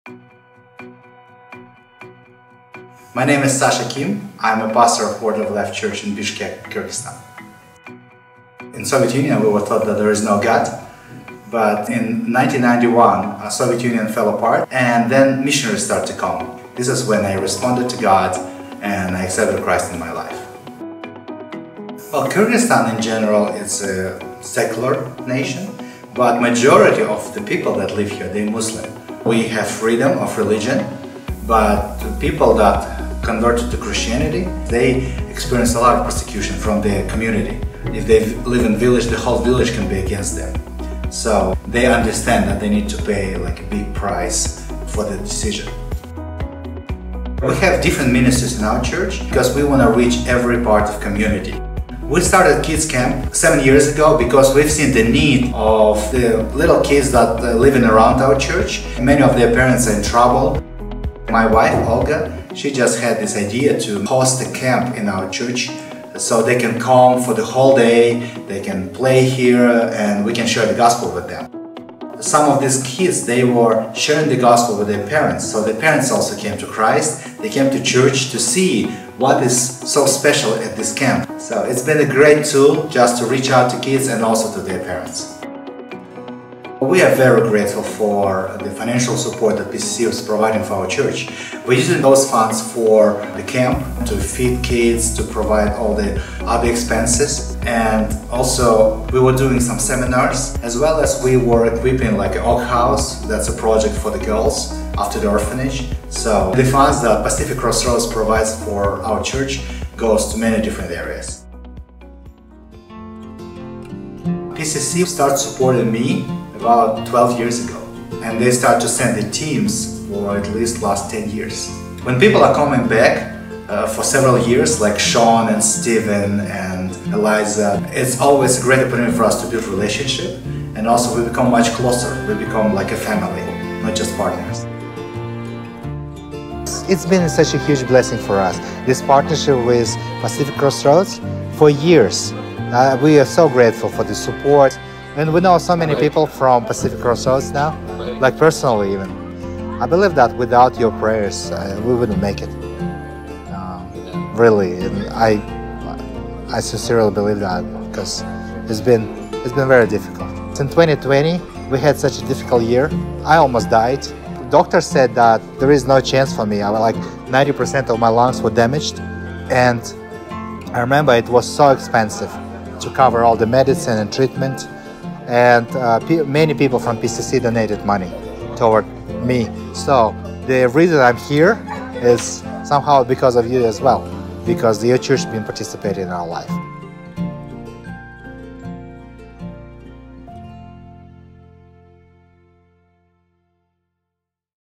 Missionary Partners: Sasha & Olga Kim
Learn more about how PCC's relationship has blessed Sasha and Olga Kim's work in Kyrgyzstan.